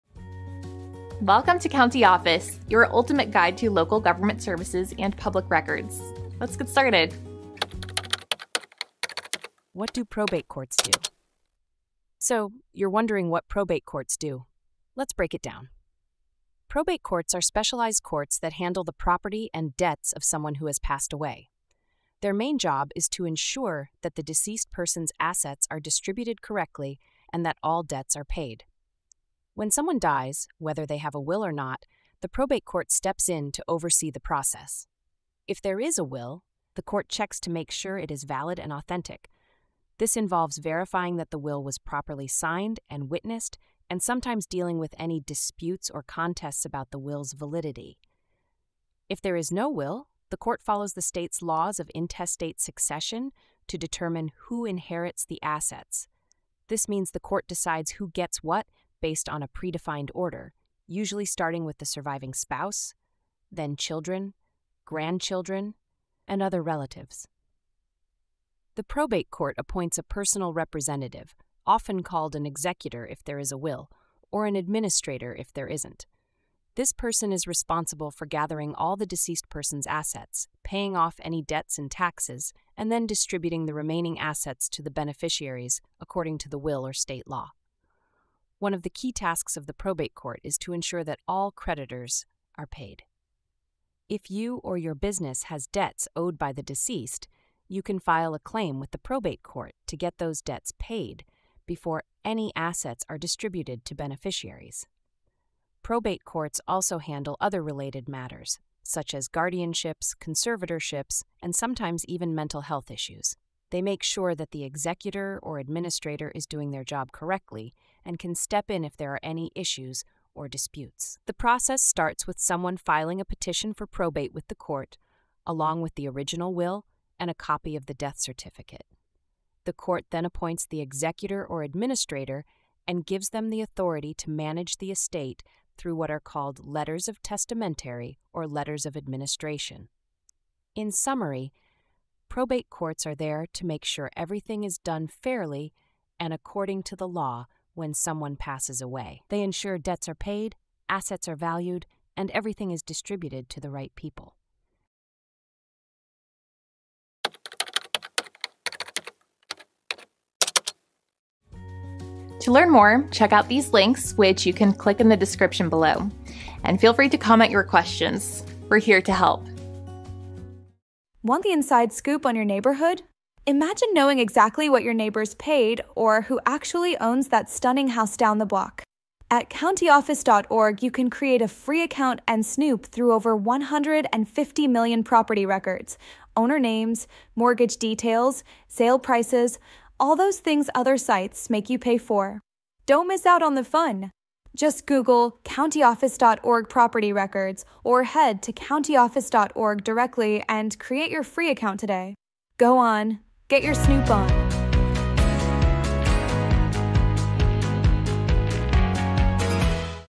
What Do Probate Courts Do? - CountyOffice.org
What Do Probate Courts Do? Probate courts play a crucial role in the legal system by managing the distribution of assets and settling debts after someone has passed away. In this informative video, we will delve into the functions of probate courts and how they ensure that the wishes of the deceased are honored, whether or not a will exists. We will explore the process of validating a will, appointing executors or administrators, and the steps involved in the probate process. Additionally, we will discuss the importance of creditor claims and how probate courts oversee the payment of debts before assets are distributed to beneficiaries.

Furthermore, we will touch upon other responsibilities of probate courts, including guardianships and conservatorships, and how they handle disputes that may arise during the probate process. Understanding the role of probate courts is essential for anyone involved in estate planning or dealing with the loss of a loved one. Join us as we break down the complexities of probate courts and provide valuable insights that can help you navigate this important legal process.